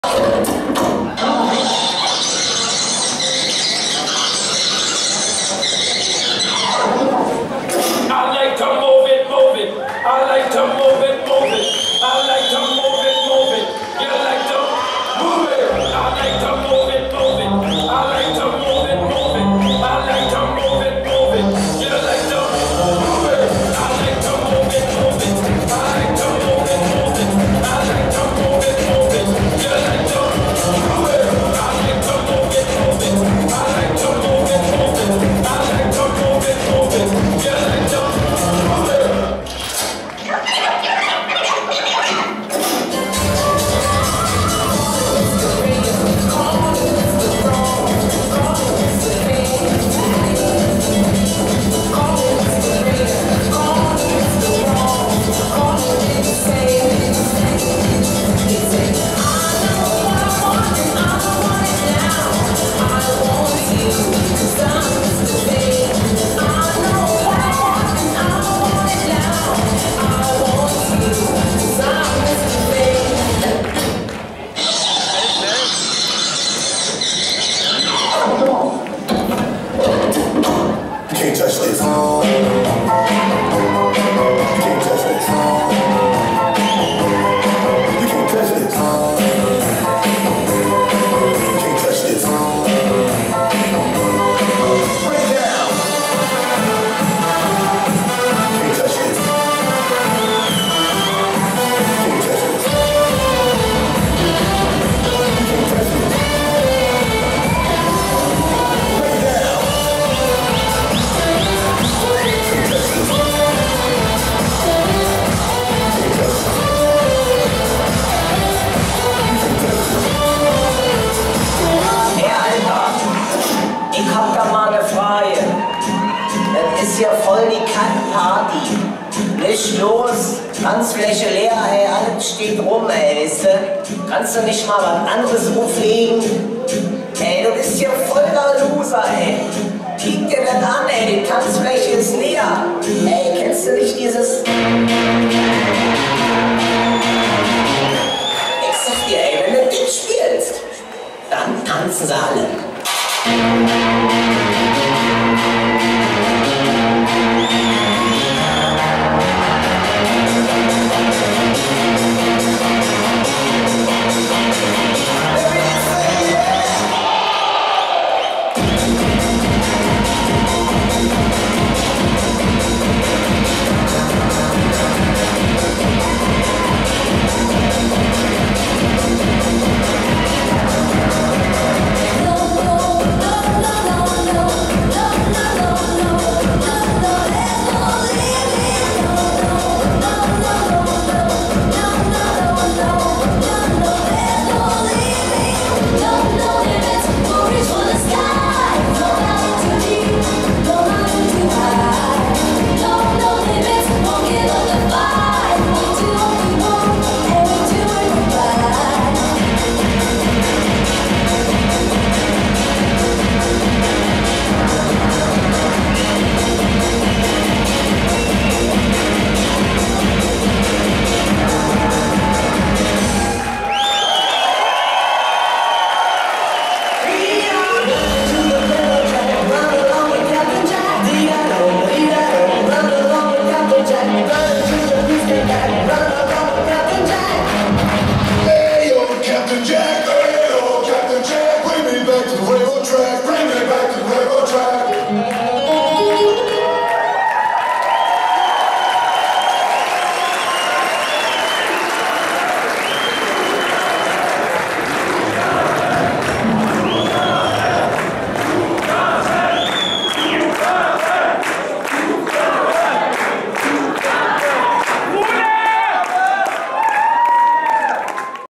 90er Tanz der Funkengarde - LCC Classics Teil 6
Ein Gute-Laune-Kracher des Lomnitzer Carnevalsclubs aus der  39. Saison 2015